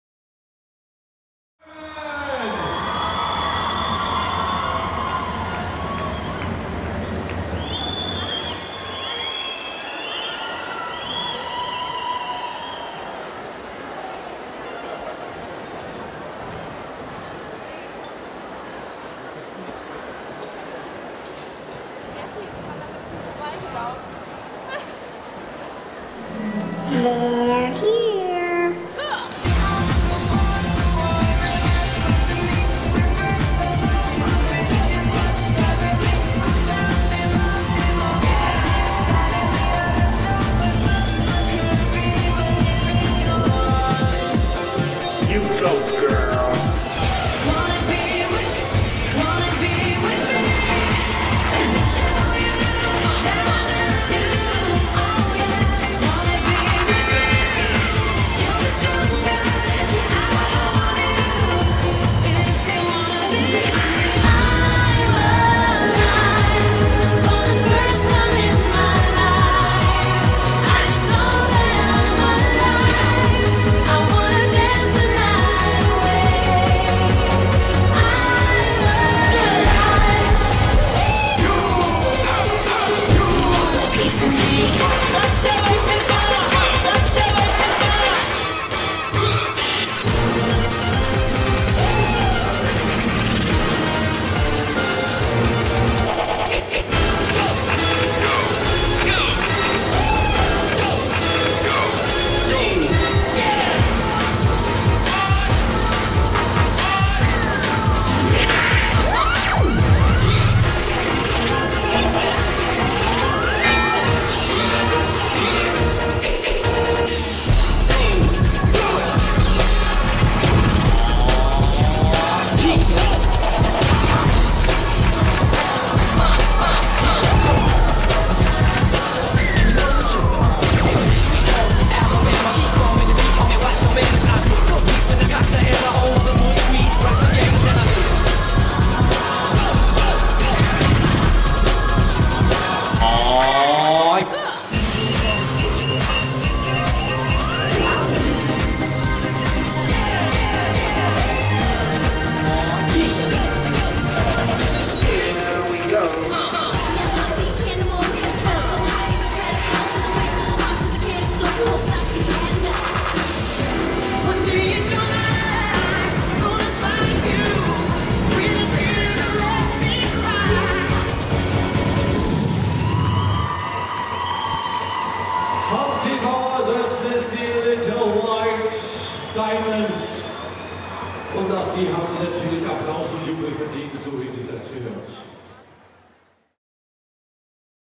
RMOst2015 - Little White Diamonds - Peewee Cheer Level 1
Little White Diamonds from Cheerleading Obervogtland  (GER) in Peewee Cheer Level 1 . - RMO 2015 - CCVD Regionalmeisterschaft Ost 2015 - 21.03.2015 SACHSENarena